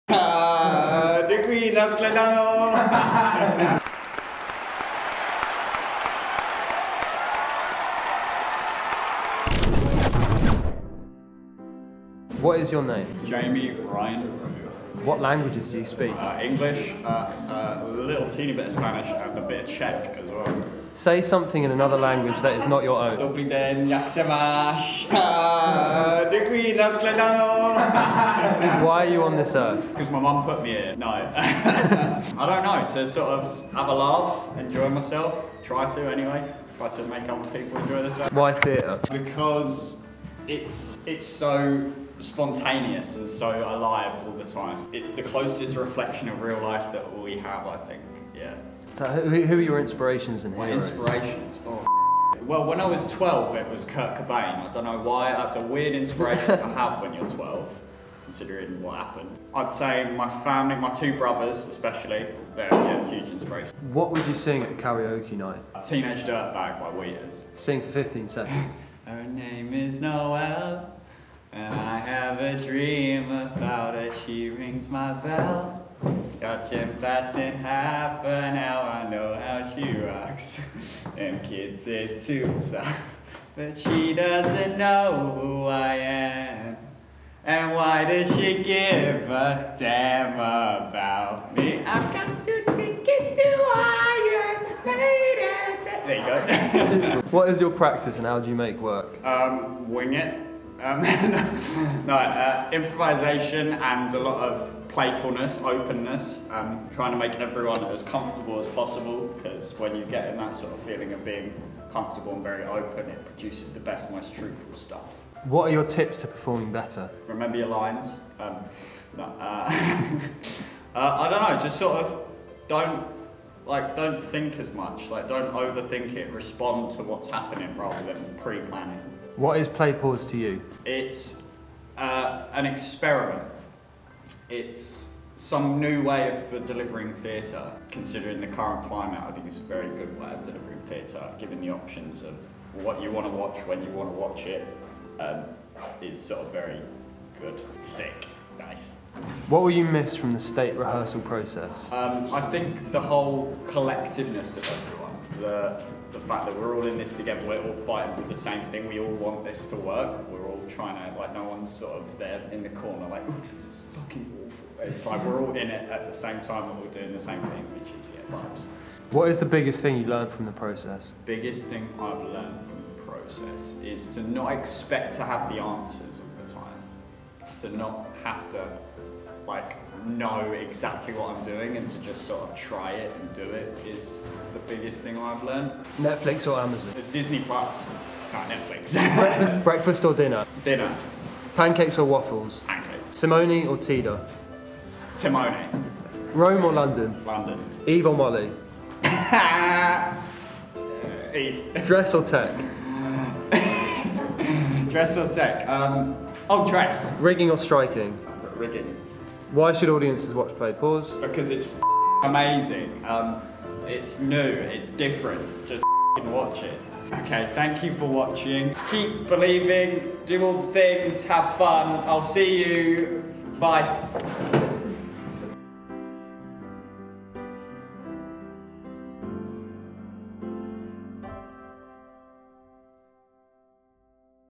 Jaime Brew on players [pause]
Episode 8 with Jaime @jaimebrew , who breaks down what it’s like in the world of making play [pause] and also gives us some vocals. Unfortunately he didn’t mention any of the businesses he owns... 🙃